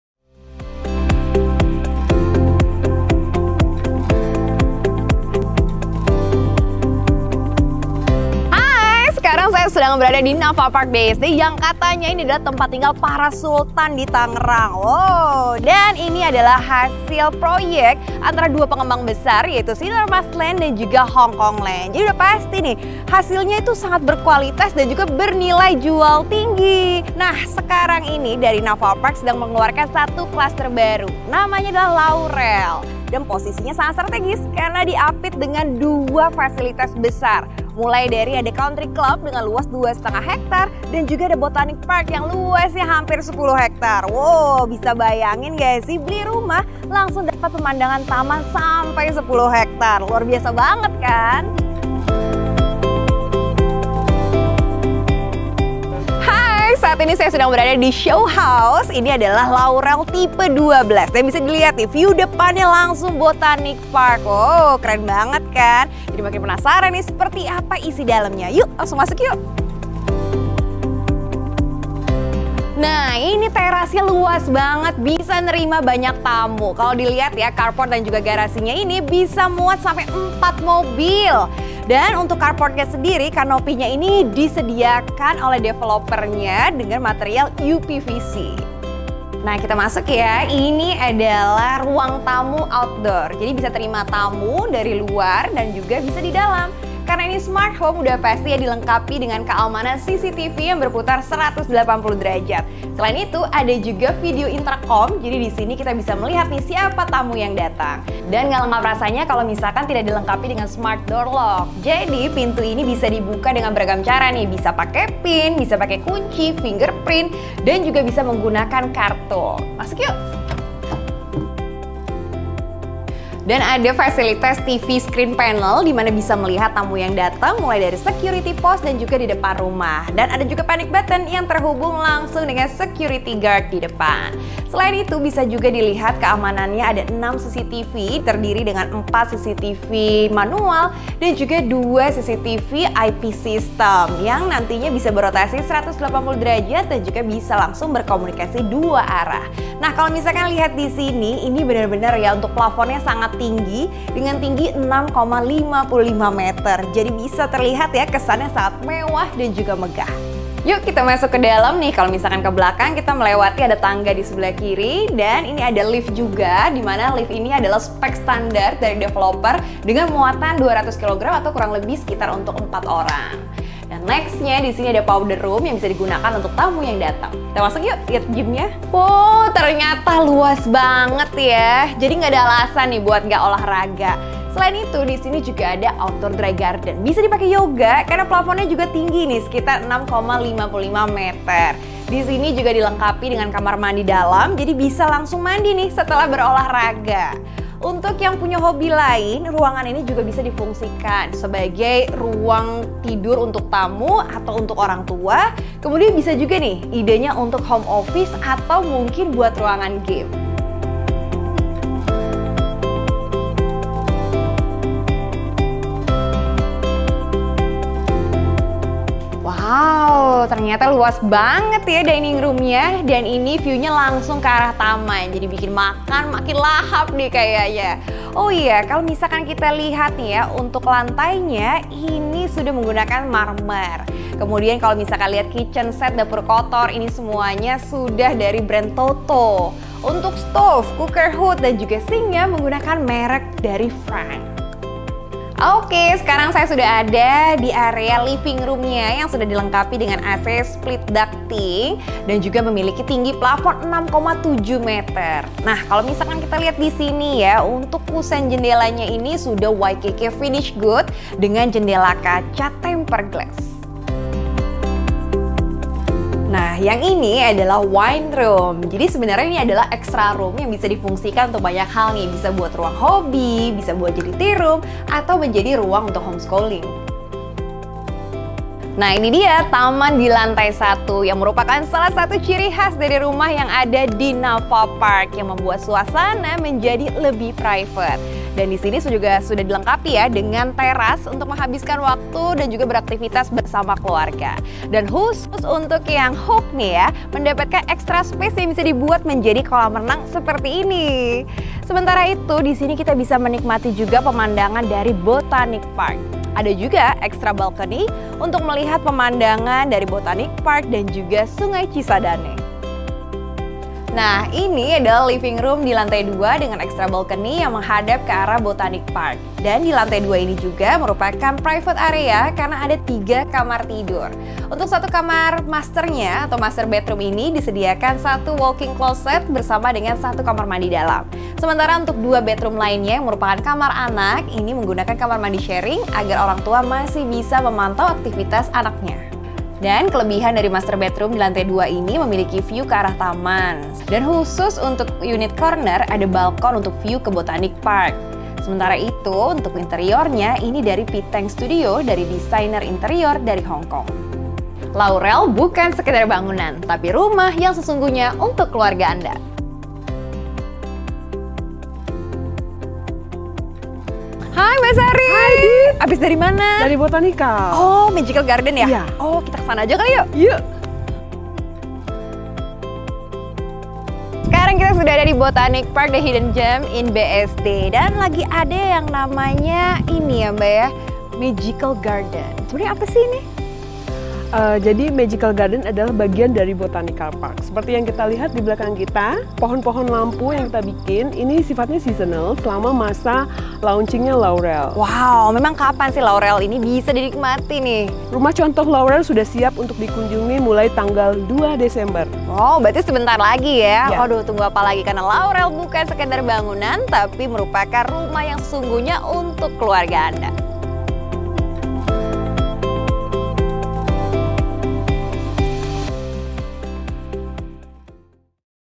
Home Tour Super premium Home Laurel Navapark BSD city type 12 with 10 acre Garden
Cluster Laurel Navapark BSD City With 10 Ha Botanical Park

Nama Project : Cluster LAUREL
Lokasi : Nava Park Bsd City
Developer : Sinarmas land & Hongkong land

Tipe 12x23
4 Kamar Tidur / 5 Kamar Mandi
1 Kamar pembantu + Kamar mandi
2 carport + 2 garasi

CARA BAYAR
- Hard cash
- Installment
- KPR Bank

Info lengkap:
Ellen Widjaja